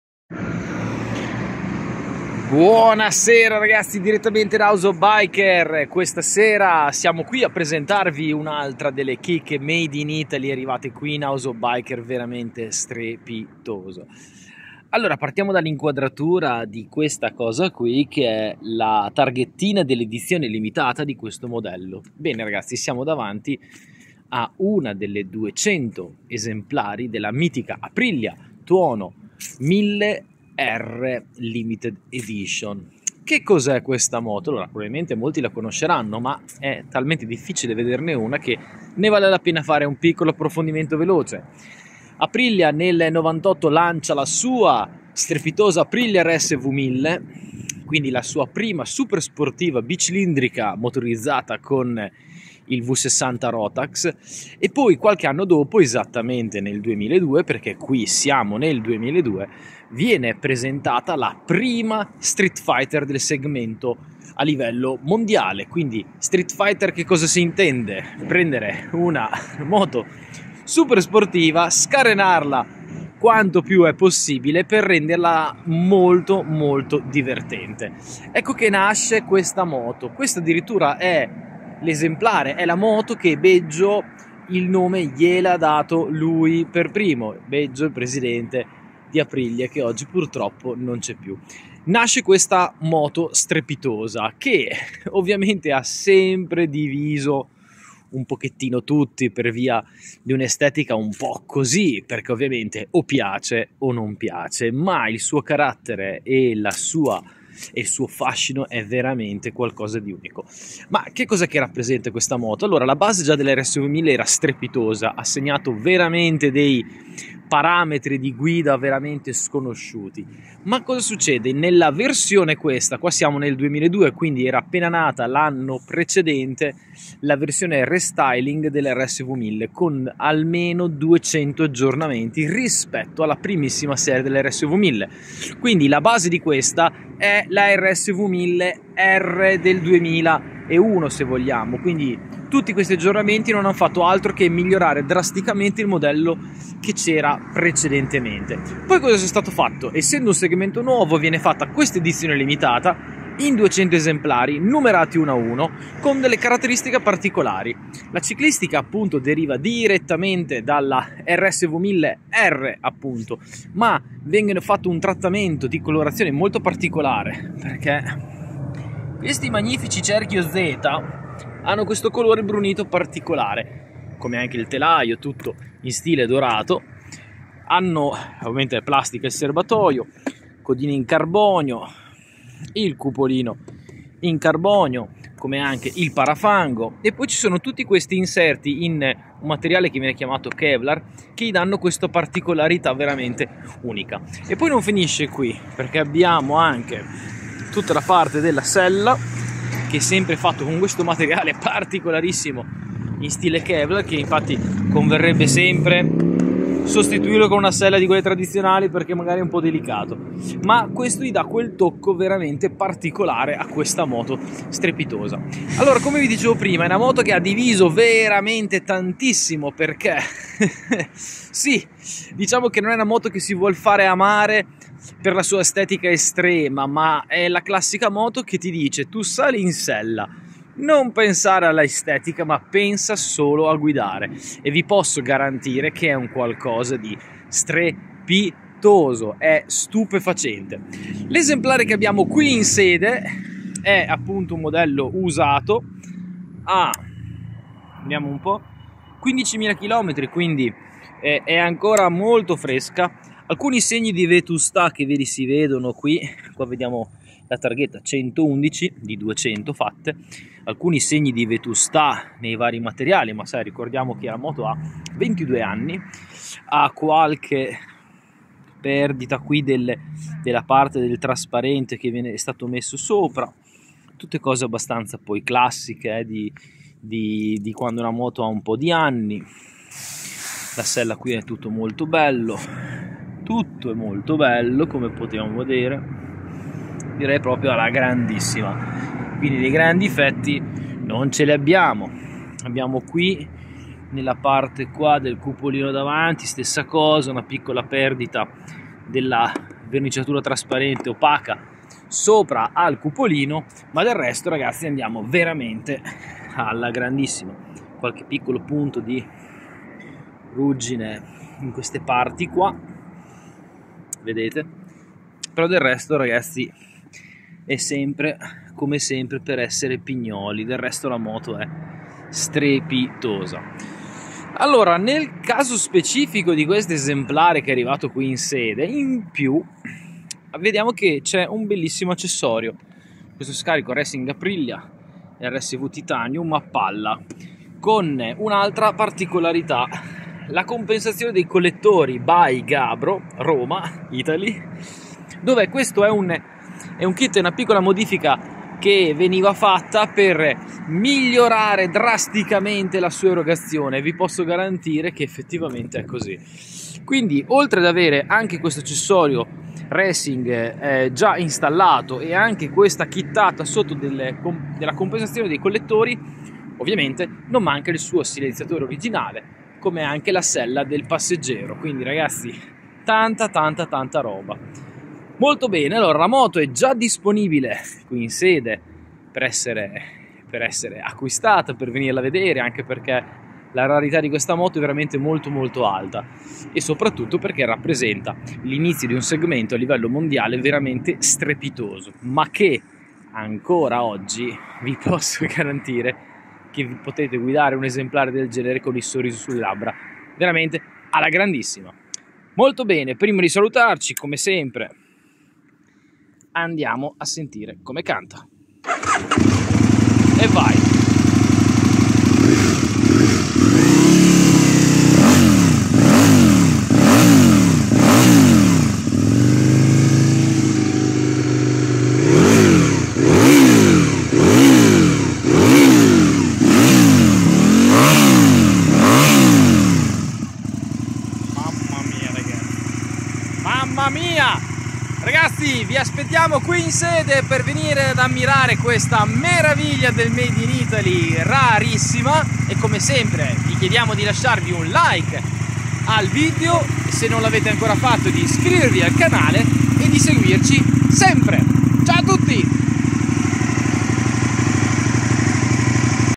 APRILIA TUONO 1000 R LIMITED disponibile alla HOB
new entry da sballo alla house of bikers

Signori e Signore. Miss Aprilia Tuono 1000 r Limited edition...

Solo 200 esemplari al mondo realizzati...

Ecco a voi l'inizio della saga delle Turbo super Street fighter a Palla !

Moto gia' disponibile in sede

PROVINCIA DI MODENA
ITALY
( MOTOR VALLEY )